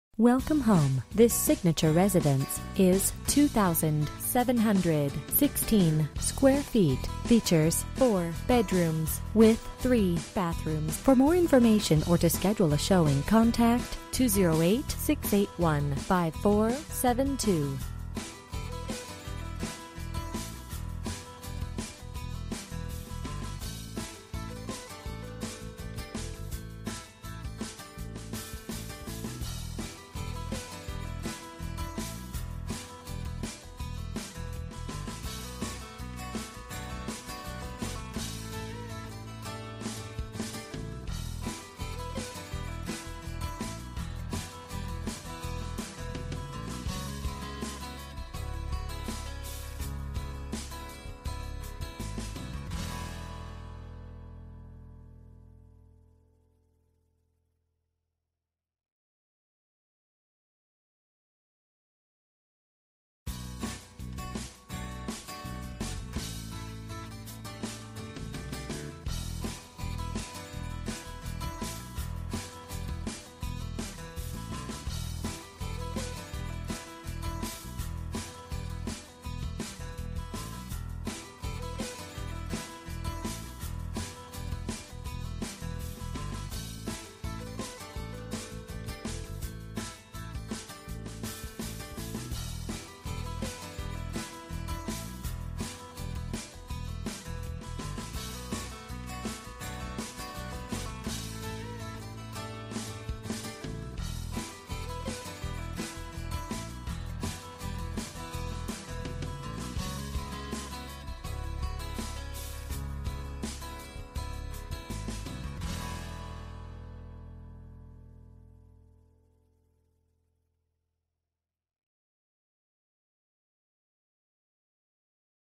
Homes for sale - 1123 Norton Avenue, IDAHO FALLS, ID 83402
This cute house is located in the desirable Johns Heights neighborhood on the West side of Idaho Falls. The 4 bedroom 3 bath home has been kept immaculate and has had some great updates. The home features large bedrooms and lots of storage space. It also includes great outdoor space including a garden area, covered patio and an RV parking area. An attic fan keeps the home cool during the summer months. Gas is also available to home. Come see this beautiful home today!

Bedrooms: 4
Bathrooms: 3 Bathrooms
Square Feet: 2716
Price: $178,000

10/29/2016 12:51:02 pm

Last modified: